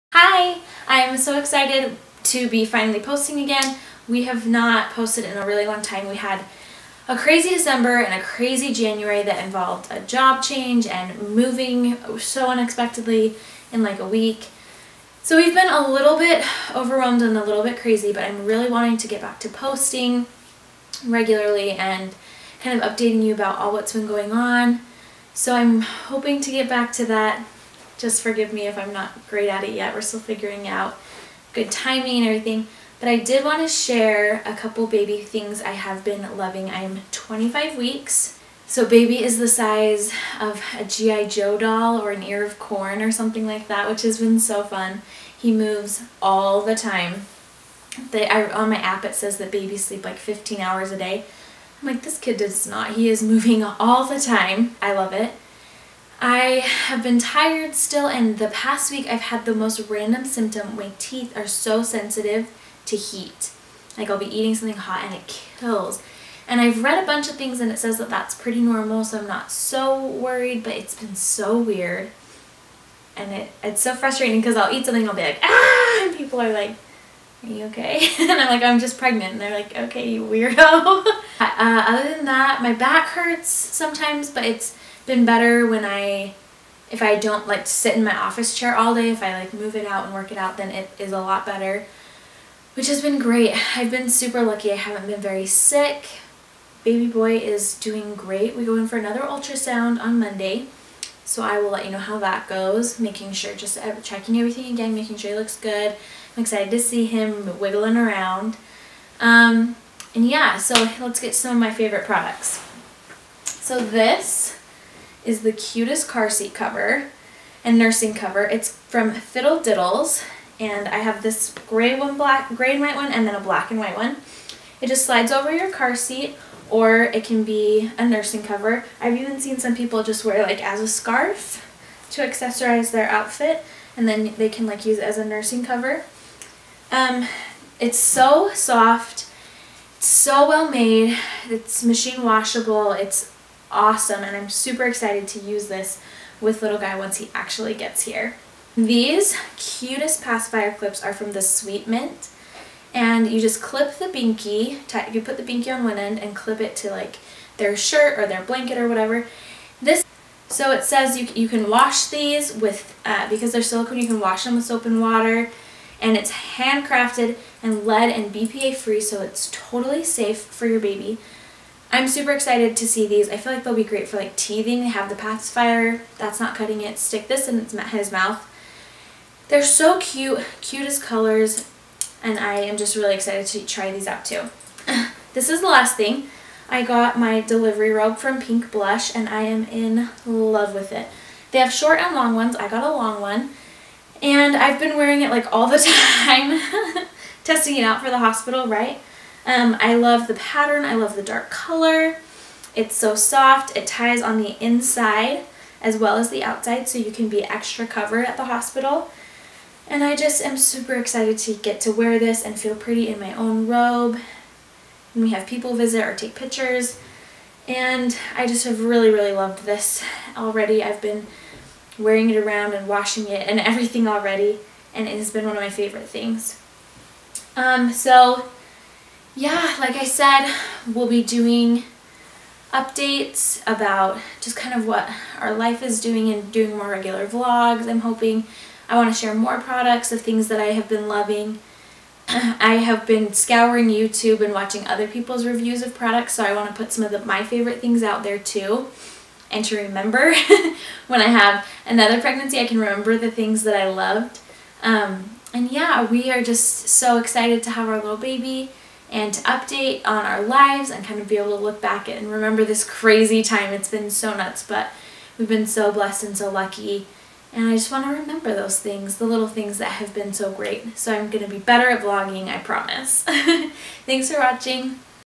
A Few Favorite Baby Things - 25 Weeks
I'm so excited to share three of my favorite baby products and a little bumpdate!